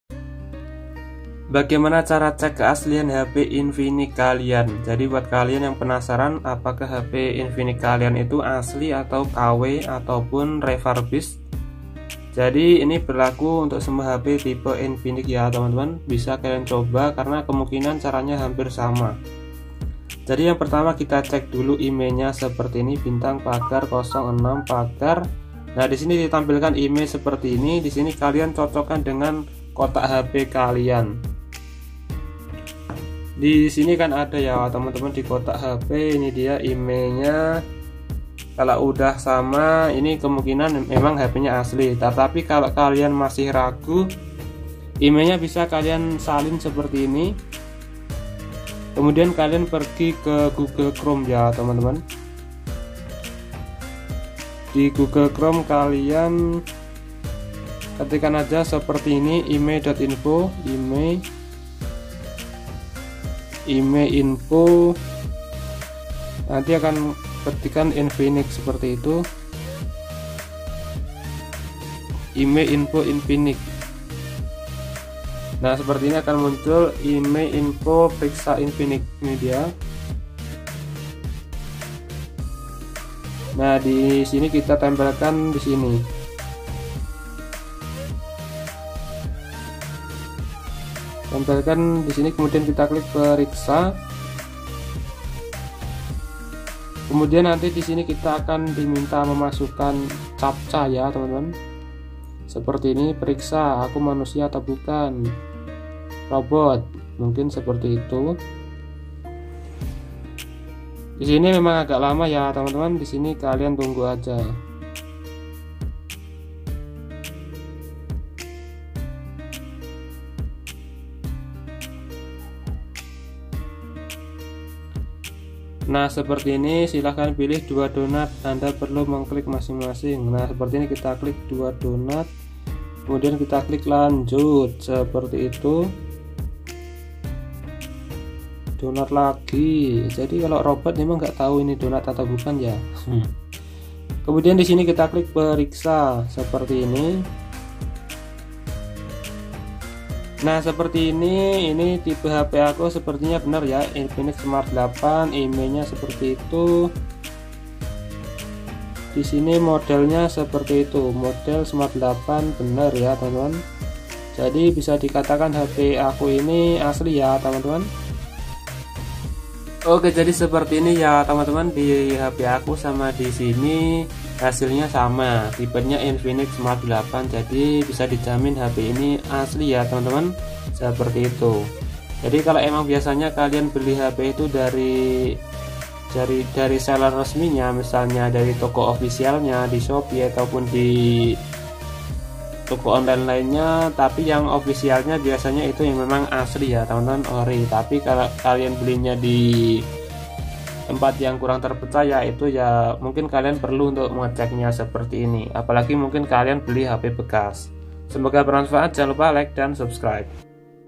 Cara cek HP infinix Asli atau Palsu | semua tipe infinix
Deskripsi:

Bosan dengan HP Infinix palsu yang beredar di pasaran?  Video ini akan membantu Anda untuk membedakan HP Infinix asli dan palsu dengan mudah. Kami akan menunjukkan cara cek IMEI, memeriksa kemasan, dan fitur-fitur lain yang perlu Anda perhatikan. Jangan sampai tertipu!